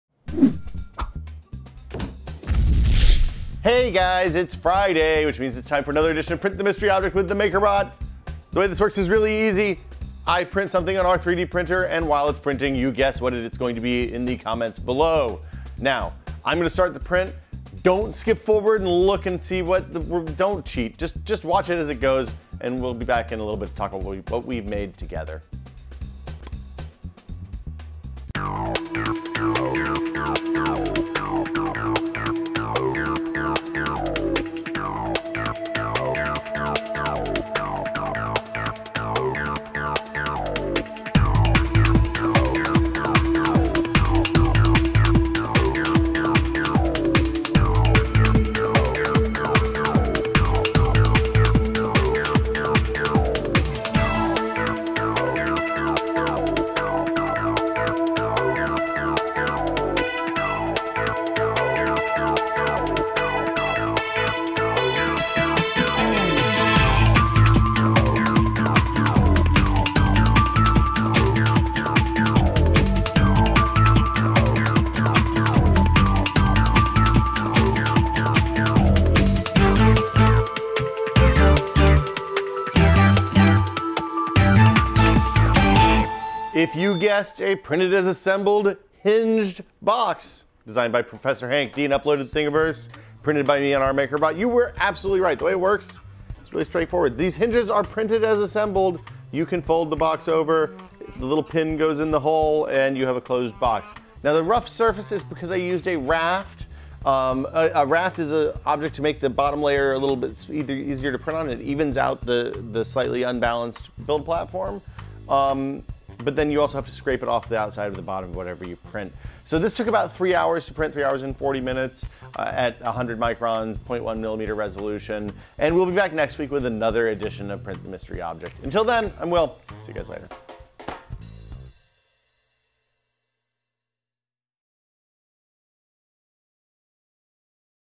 MakerBot Mystery Build: Smart Cookie
It's Friday, which means it's time for another MakerBot Mystery Build! This week's print demonstrates one of the unique qualities of objects you can make with the Replicator. Place your best guess as to what's being 3D printed in the comments below.